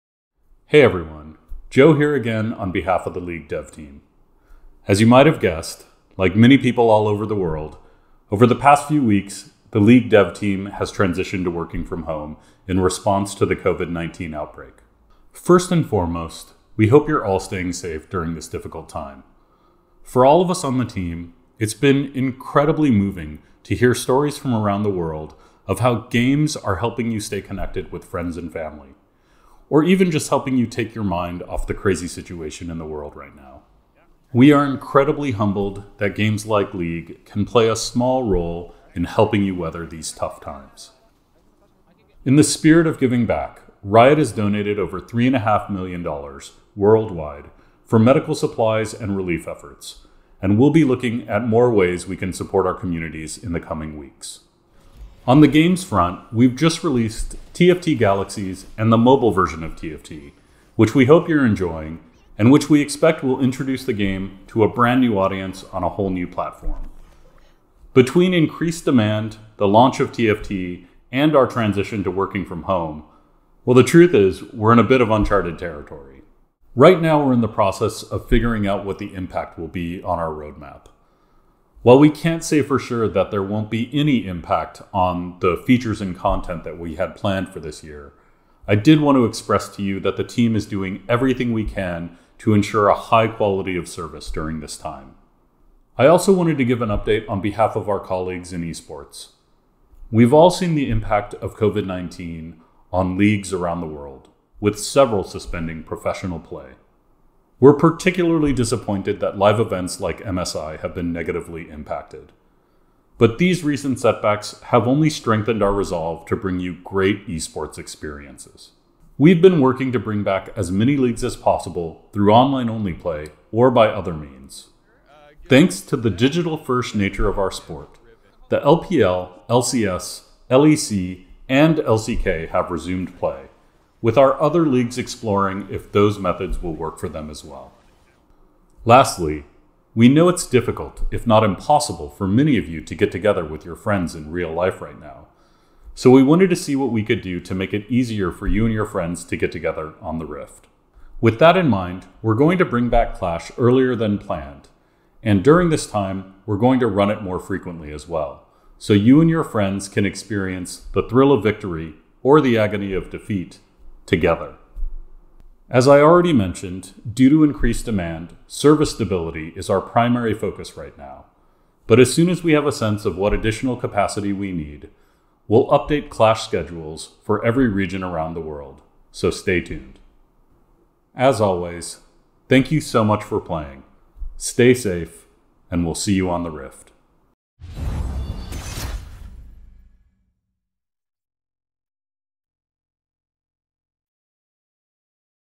LoL 개발팀에서 드리는 메시지 | 개발자 영상 - 리그 오브 레전드
리드 프로듀서 “New001” 조가 코로나 사태와 관련하여 LoL, e스포츠 등의 상황을 알려드립니다.